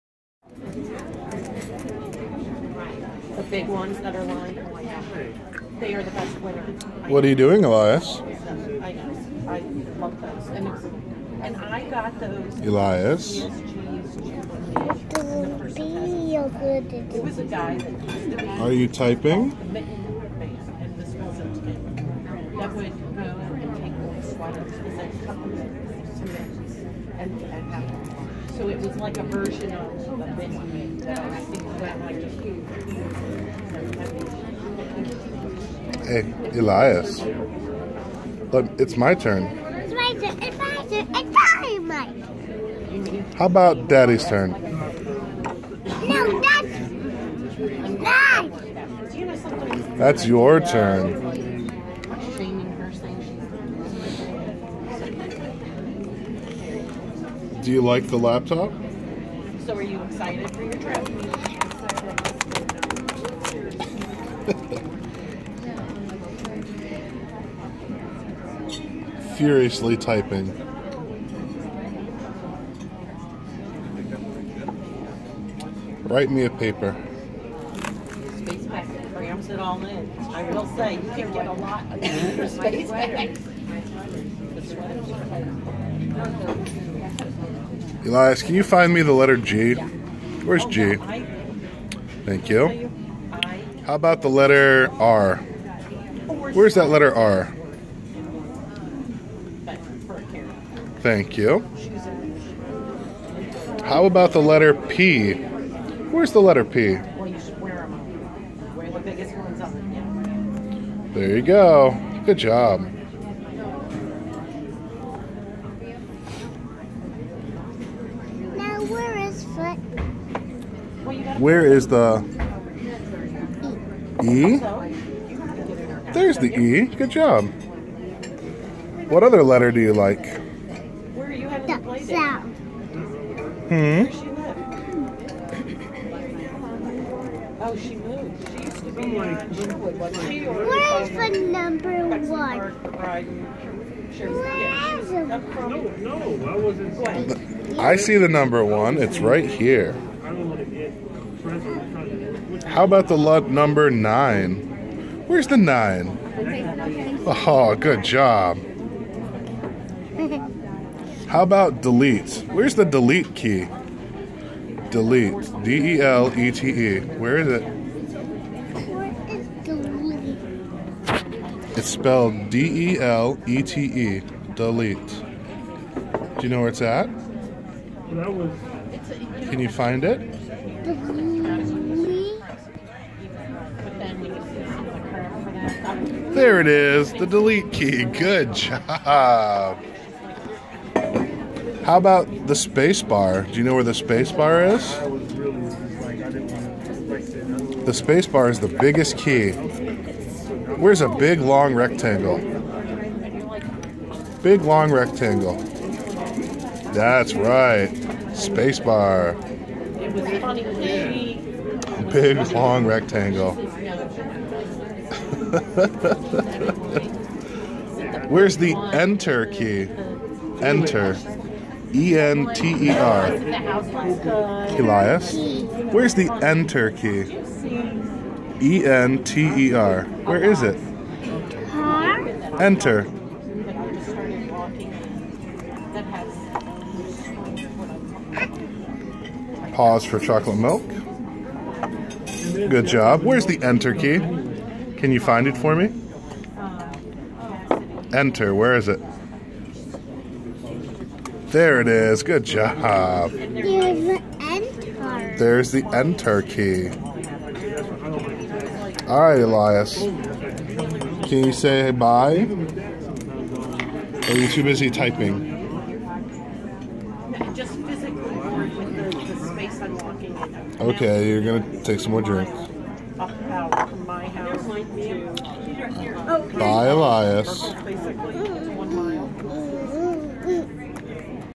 Elias Typing Away on the Laptop
2 and a half year old Elias displaying his L33T typing skills for Daddy.